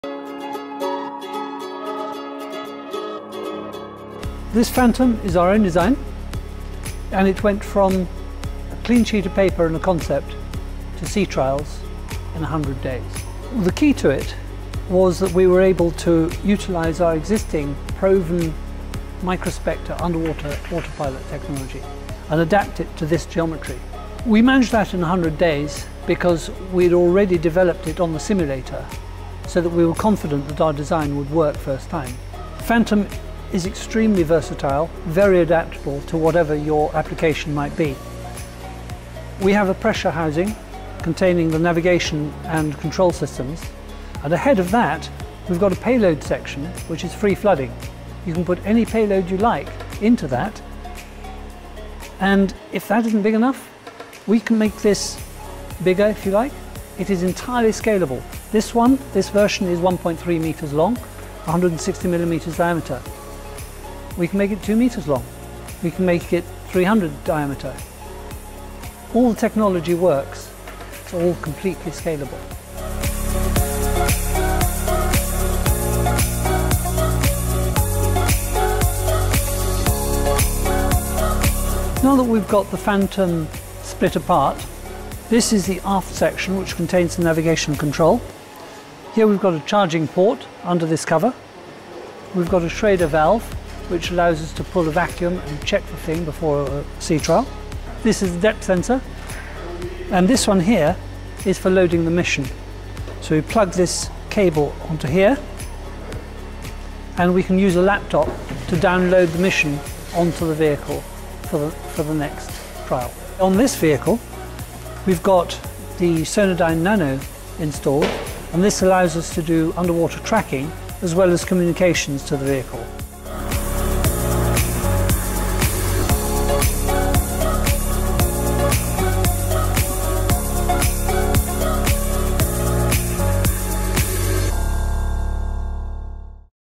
From Concept to Product in 100 days - 'How to' design a UUV - Dynautics Phantom UUV
When customers demand a product in double quick time, you have to step up and take the challenge. That is what happened when the US Navy wanted a specific UUV and approached Dynautics.
Some customers you just can't say 'no' to, so we took on the challenge.
The US Navy, being the US Navy, had a very tight specification and we had the simulation software (AUV SIM) and proven control and guidance technology with the Underwater Spectre firmware and hardware to make it a fair fight. Combining these tools, technologies and rapid prototyping techniques and the products were delivered, tested and accepted within 100 days, start to finish!
We are now offering the same service to others, including a fully customisable design to suit customers' mission requirements.. Let us know your mission.